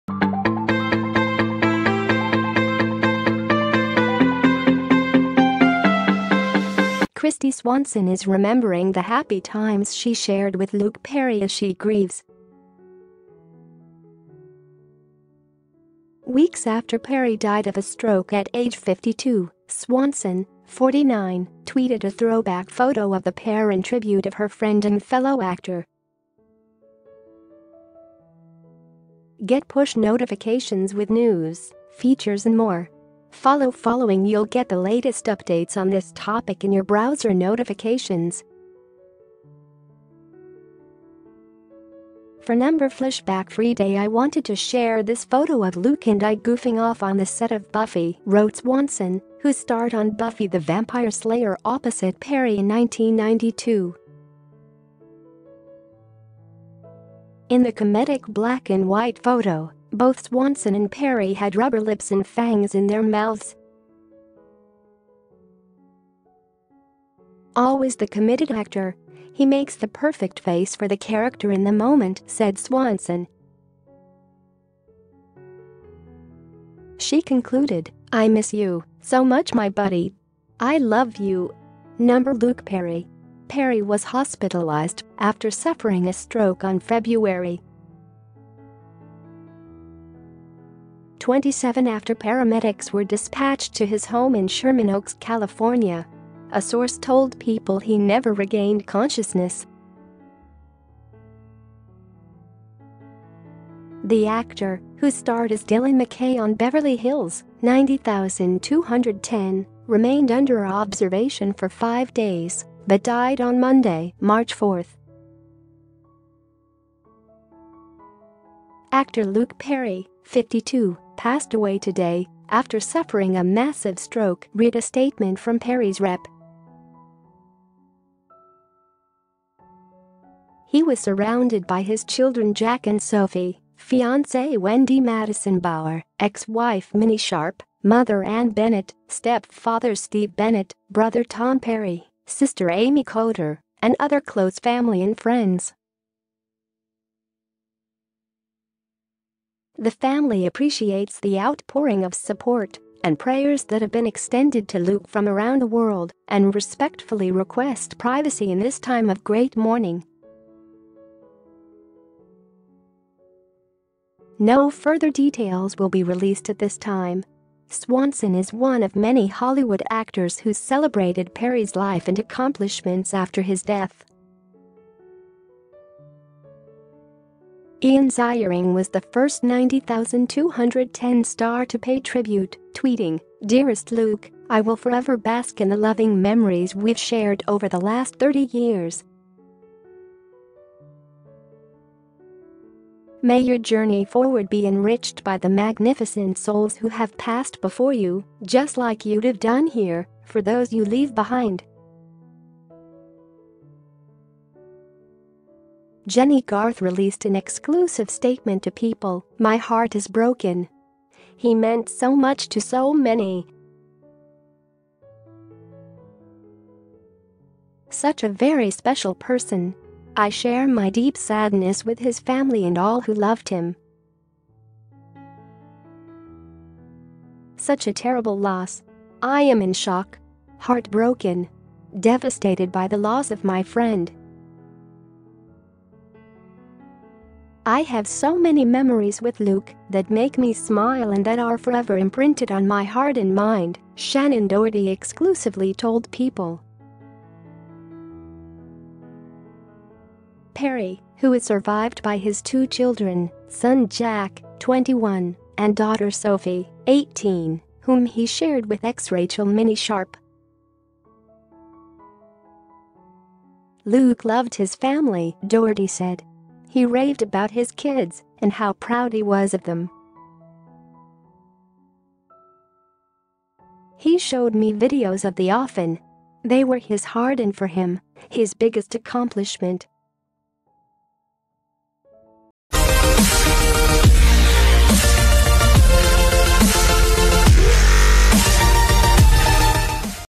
Buffy Star Kristy Swanson Shares Throwback Photo of Luke Perry: 'I Miss You So Much' - News 247
Kristy Swanson is remembering the happy times she shared with Luke Perry as she grieves.  Weeks after Perry died of a stroke at age 52, Swanson, 49, tweeted a throwback photo of the pair in tribute of her friend and fellow actor. Get push notifications with news, features and more. Follow Following You'll get the latest updates on this topic in your browser notifications.  “For FlashbackFriday I wanted to share this photo of Luke and I goofing off on the set of Buffy,” wrote Swanson, who starred on Buffy the Vampire Slayer opposite Perry in 1992.  In the comedic black-and-white photo, both Swanson and Perry had rubber lips and fangs in their mouths.  “Always the committed actor, he makes the perfect face for the character in the moment,” said Swanson.  She concluded, “I miss you so much my Buddy. I love you. LukePerry.”  Perry was hospitalized after suffering a stroke on Feb. 27 after paramedics were dispatched to his home in Sherman Oaks, California. A source told PEOPLE he never regained consciousness.  The actor, who starred as Dylan McKay on Beverly Hills, 90210, remained “under observation” for five days, but died on Monday, March 4.  “Actor Luke Perry, 52, passed away today after suffering a massive stroke,” read a statement from Perry’s rep. “He was surrounded by his children Jack and Sophie, fiancé Wendy Madison Bauer, ex-wife Minnie Sharp, mother Ann Bennett, step-father Steve Bennett, brother Tom Perry, sister Amy Coder, and other close family and friends. The family appreciates the outpouring of support and prayers that have been extended to Luke from around the world, and respectfully request privacy in this time of great mourning. No further details will be released at this time.”  Swanson is one of many Hollywood actors who celebrated Perry’s life and accomplishments after his death.  Ian Ziering was the first 90210 star to pay tribute, tweeting, “Dearest Luke, I will forever bask in the loving memories we’ve shared over the last thirty years. May your journey forward be enriched by the magnificent souls who have passed before you, just like you have done here, for those you leave behind.”  Jennie Garth released an exclusive statement to PEOPLE: “My heart is broken. He meant so much to so many. Such a very special person. I share my deep sadness with his family and all who loved him. Such a terrible loss.”  “I am in shock. Heartbroken. Devastated by the loss of my friend. I have so many memories with Luke that make me smile and that are forever imprinted on my heart and mind,” Shannen Doherty exclusively told PEOPLE.  Perry, who is survived by his two children,  son Jack, 21, and daughter Sophie, 18, whom he shared with ex Rachel “Minnie” Sharp.  “Luke loved his family,” Doherty said. “He raved about his kids and how proud he was of them. He showed me videos of the often. They were his heart and for him, his biggest accomplishment.” Buffy Star Kristy Swanson Shares Throwback Photo of Luke Perry: 'I Miss You So Much' - News 247